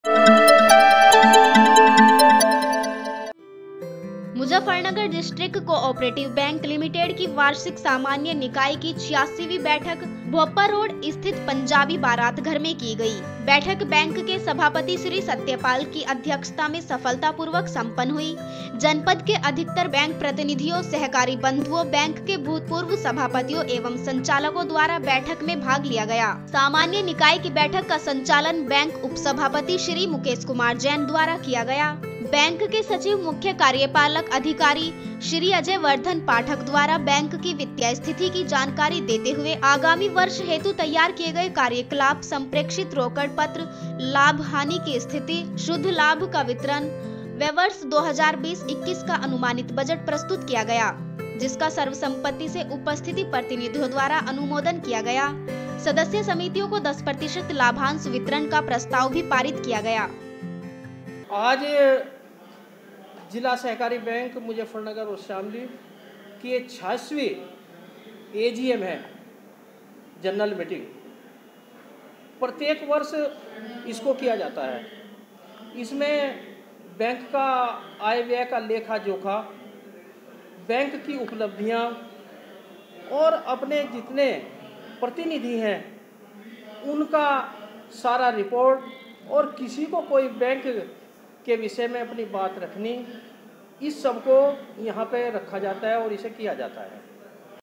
मुजफ्फरनगरः DCB Limited की 86 वीं बैठक आयोजित by samachar today
मुजफ्फरनगर डिस्ट्रिक्ट कोआपरेटिव बैंक लिमिटेड की वार्षिक सामान्य निकाय की 86वीं बैठक भोपा रोड स्थित पंजाबी बारात घर मे की गई। बैठक बैंक के सभापति श्री सत्यपाल सिह पाल की अध्यक्षता में सफलतापूर्वक सम्पन्न हुई। जनपद के अधिकतर बैंक प्रतिनिधियों , सहकारी बन्धुओं, बैंक के भूतपूर्व सभापतियों एवं संचालकों द्वारा बैठक में भाग लिया गया । सामान्य निकाय की बैठक का संचालन बैंक उपसभापति श्री मुकेश कुमार जैन द्वारा किया गया बैंक के सचिव / मुख्यकार्यपालक अधिकारी श्री अजय वर्धन पाठक द्वारा बैंक की वित्तीय स्थिति की जानकारी देते हुए आगामी वर्ष हेतु तैयार किये गये कार्यकलाप सम्प्रेक्षित रोकड पत्र , लाभ हानि की स्थिति , शुद्ध लाभ का वितरण व वर्ष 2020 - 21 का अनुमानित बजट प्रस्तुत किया जिसका सर्वसम्मति से उपस्थिति प्रतिनिधियों द्वारा अनुमोदन किया गया । सदस्य समितियों को 10 प्रतिशत लाभांश वितरण का प्रस्ताव भी पारित किया गया।